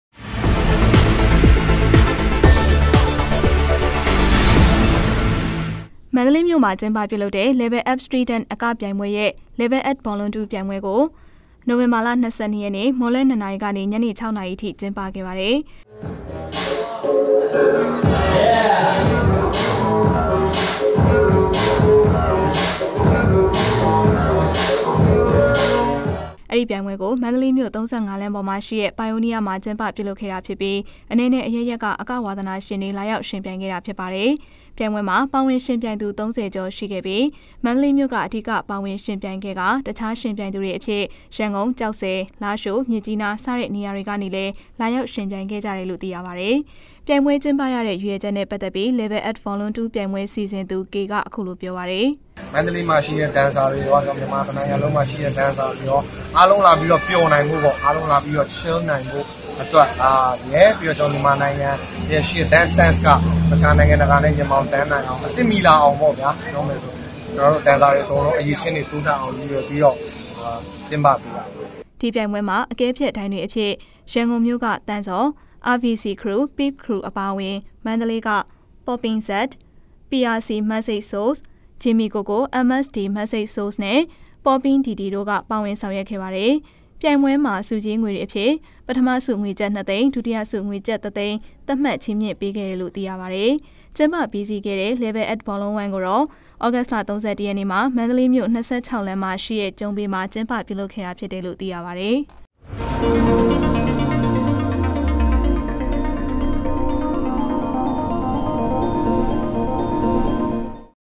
Level Up Street Dance အကၿပိဳင္ပြဲ၏ Level Up Vol 2 ၿပိဳင္ပြဲက်င္းပ
မႏၱေလးၿမိဳ႕မွာက်င္းပျပဳလုပ္တဲ့ Level Up Street Dance အကၿပိဳင္ပြဲရဲ႕ Level Up Vol 2 ၿပိဳင္ပြဲကို ႏို၀င္ဘာလ ၂၂ ရက္ေန႕က မြန္းလြဲ ၂ နာရီကေန ညေန ၆ နာရီအထိ က်င္းပခဲ့ပါတယ္။